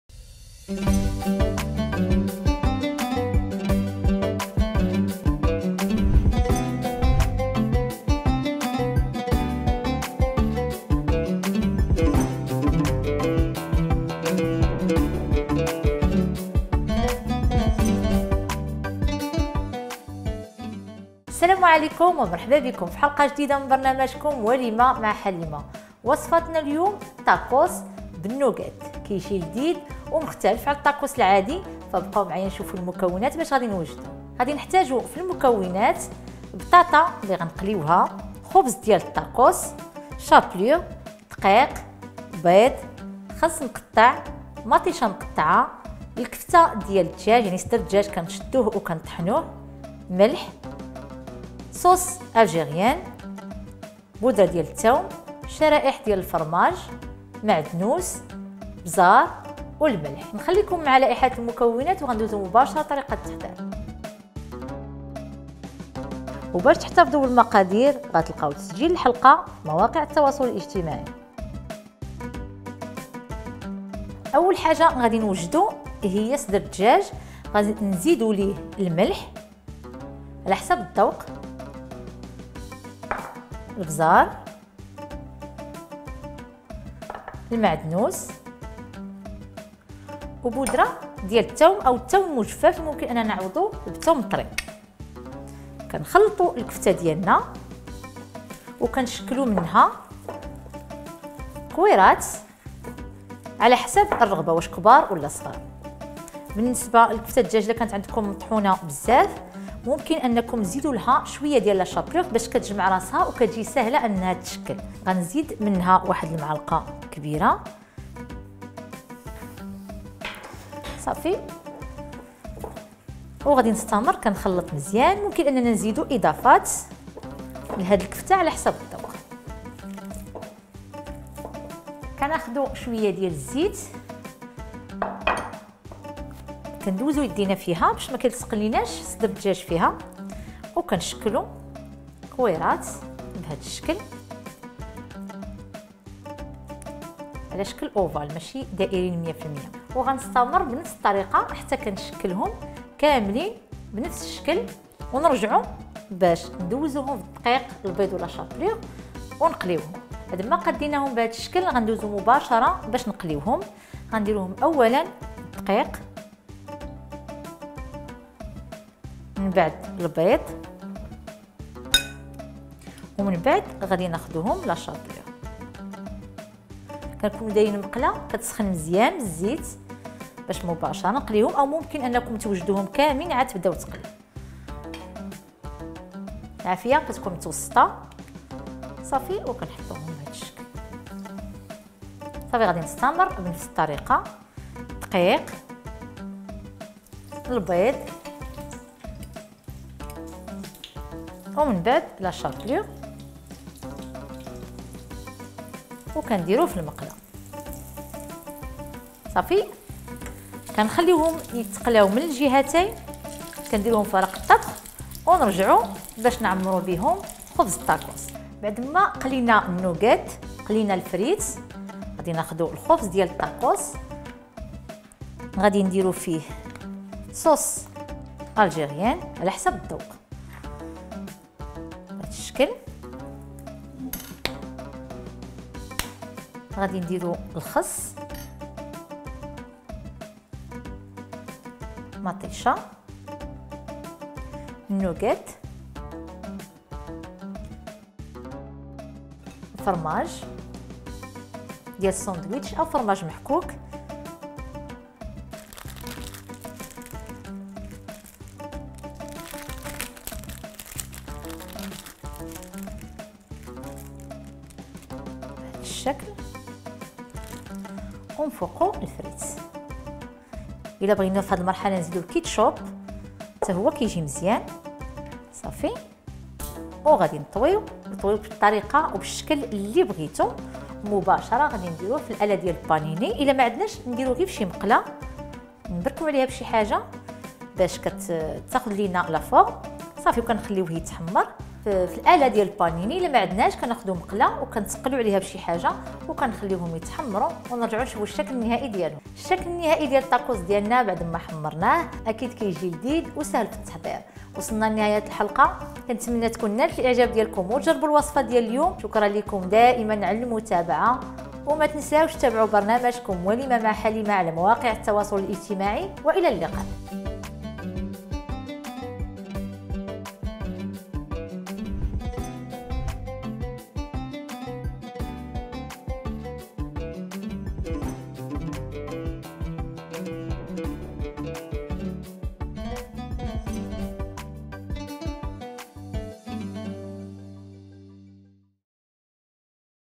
وصفة طاكوس نوكيت
مرحبا بكم في حلقة جديدة من برنامجكم وليمة مع حليمة! ❤️

الشهيوة لي غانحضرو اليوم هي وصفة طاكوس نوكيت بطريقة مبتكرة،

إكتاشفو معنا مراحل التحضير مبسطة جدا لنجاح الشهيوة لأي سيدة جرباتها!